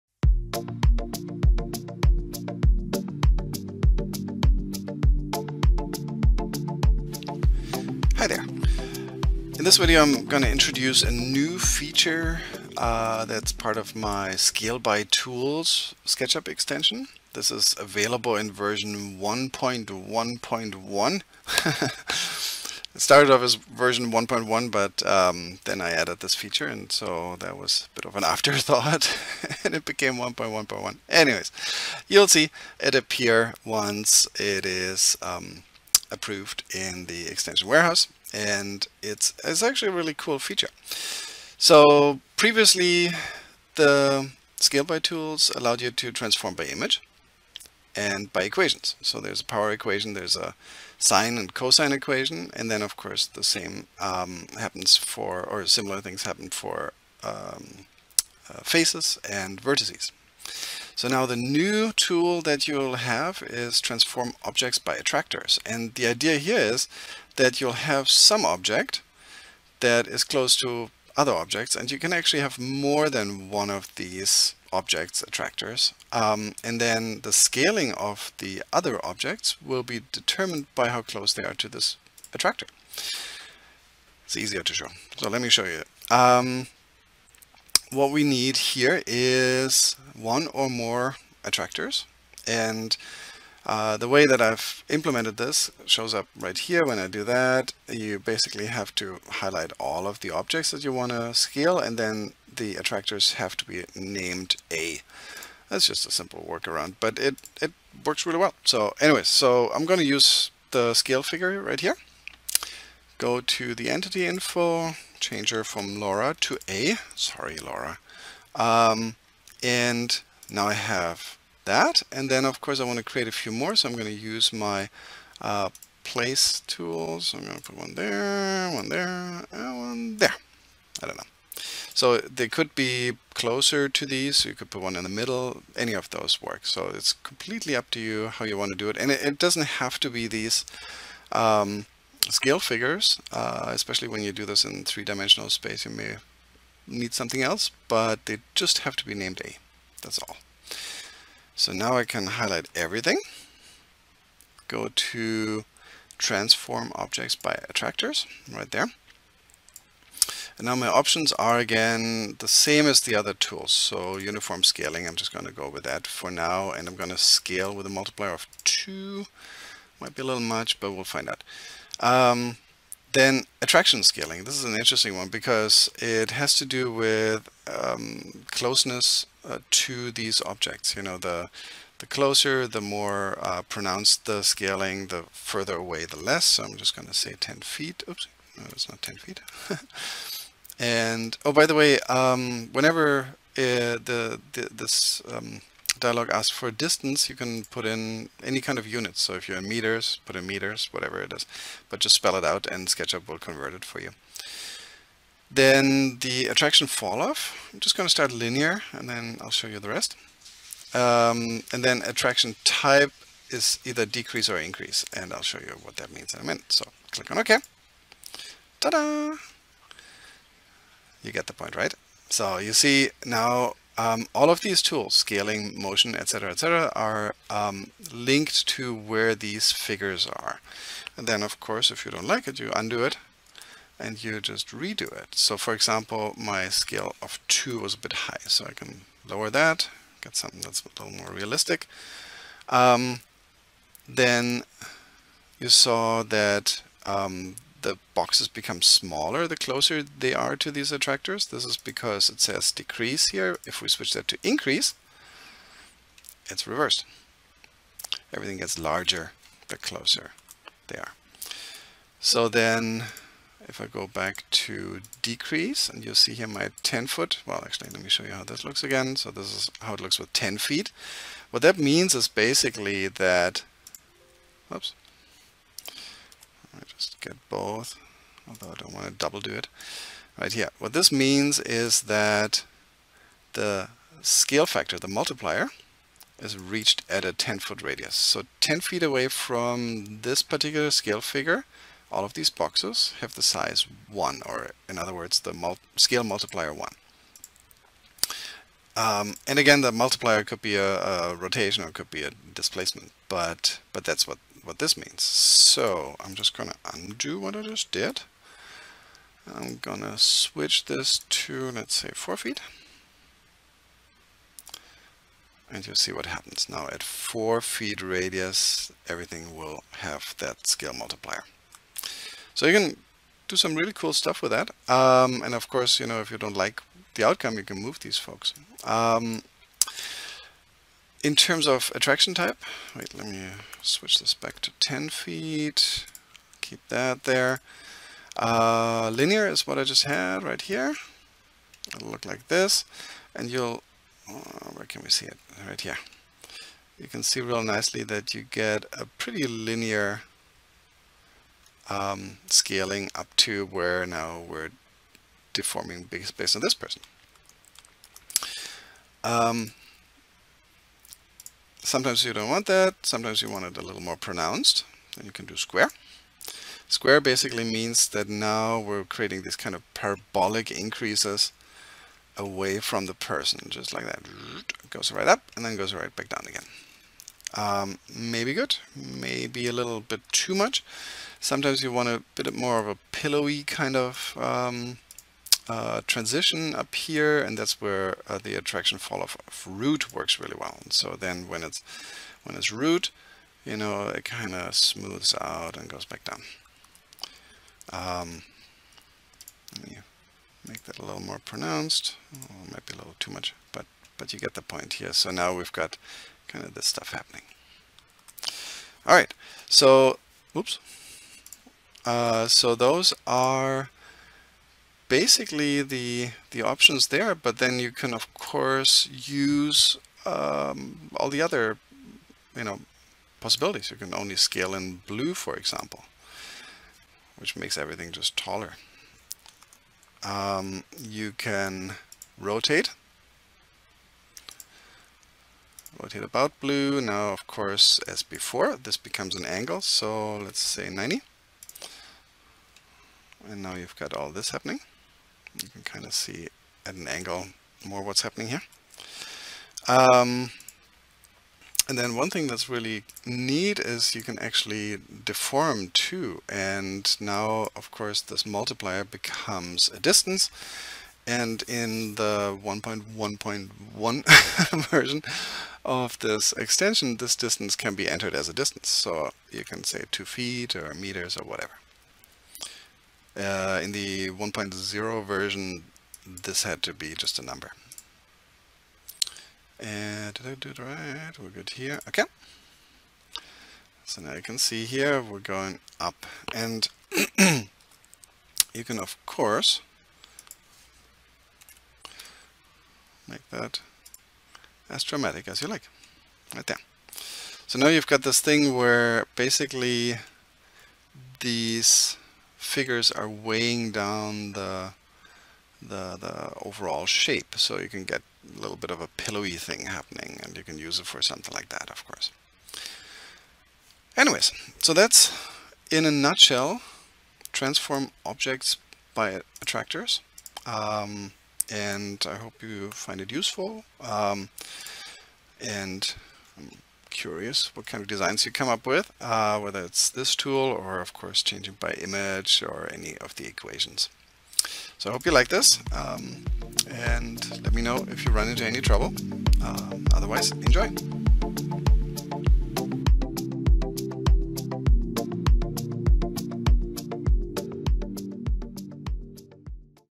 Create Beautiful Parametric Patterns with Attractor-Based Scaling (Scale By Tools Extension)
This tutorial illustrates a new feature in this extension where you can use attractors (which can be any component) to modify geometry in your model. This allows you to create parametric patterns that respond to one or several modifiers (attractors) in your 3D modeling space in SketchUp.

For more on what you can do with SketchUp, check out my and my book "Architectural Design with SketchUp".

Chapters:
0:00 Introduction and examples
1:00 New tool: Transform objects by attractors
1:33 Defining the attractors
2:40 Options for object scaling transformations
7:15 Other transformation options: scaling, rotation, motion